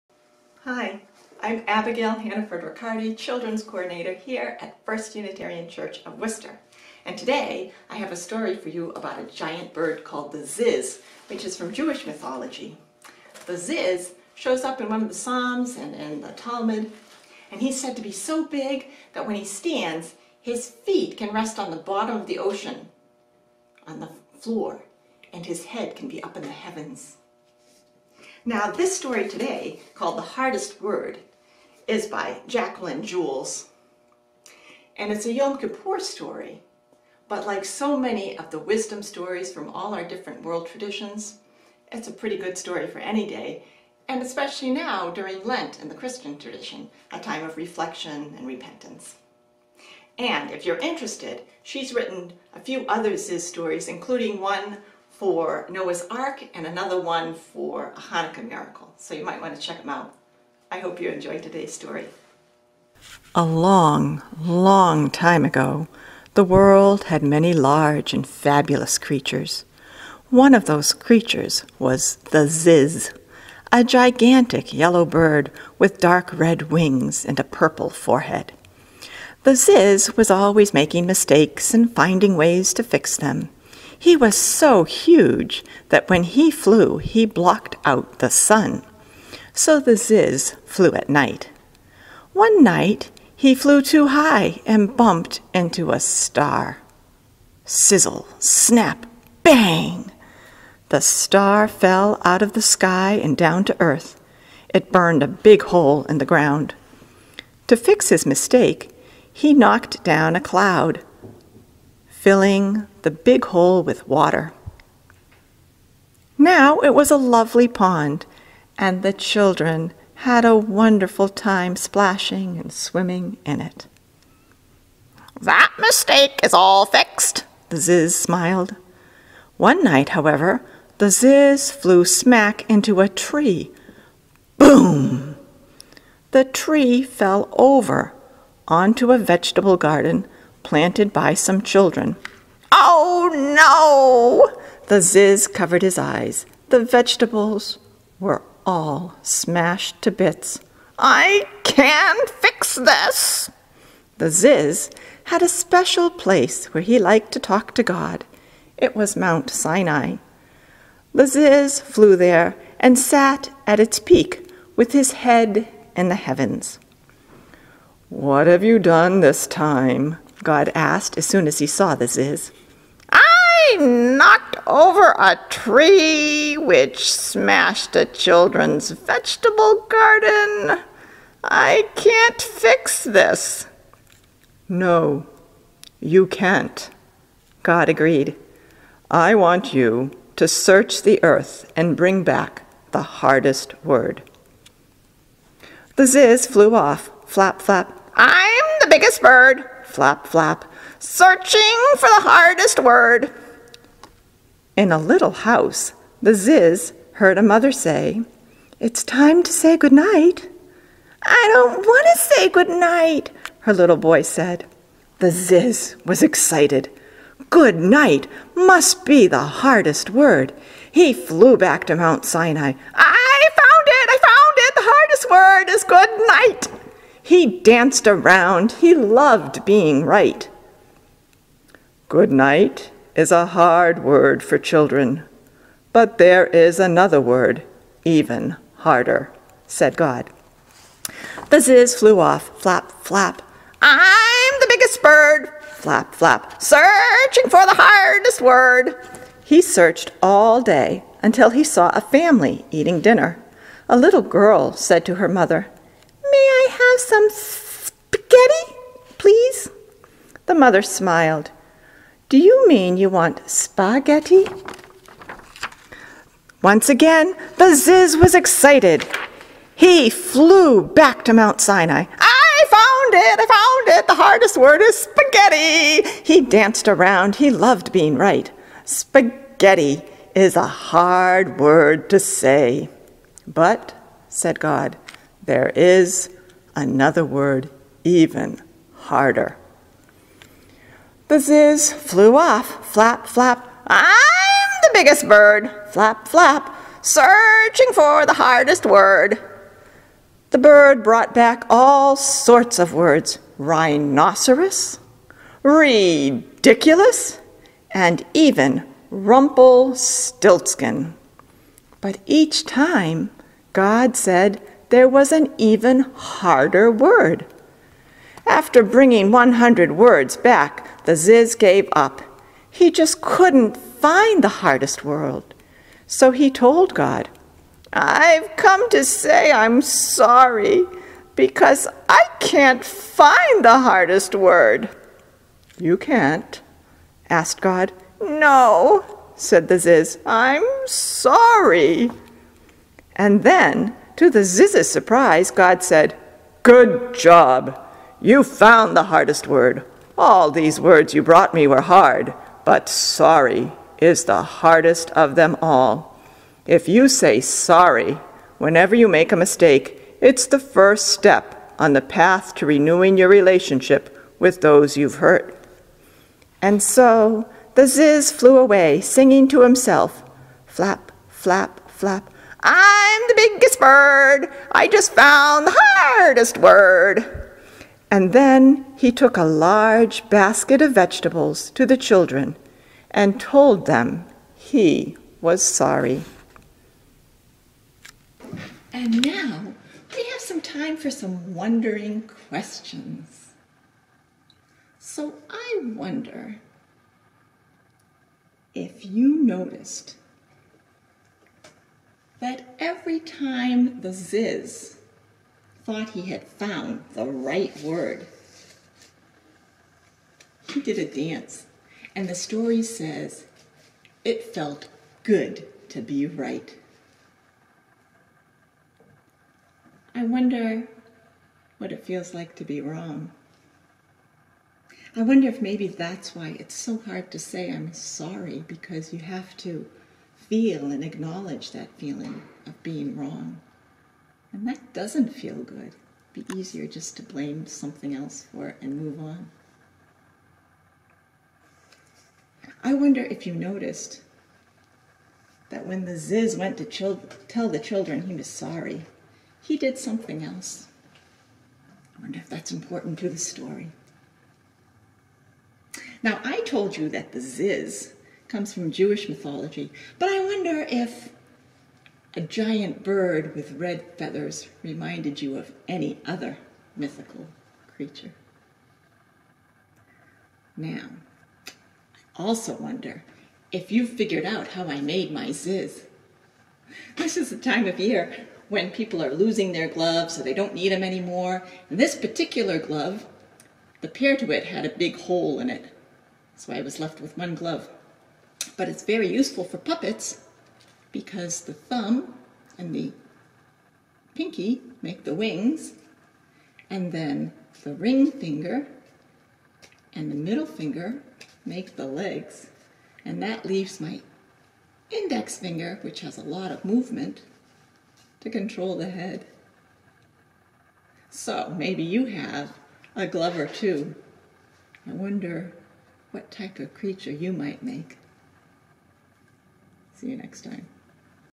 The Hardest Word
Abigail Hannaford-Ricardi, Children's Coordinator at First Unitarian Church of Worcester, tells a Spirit Play story called, "The Hardest Word" by Jaqueline Jules.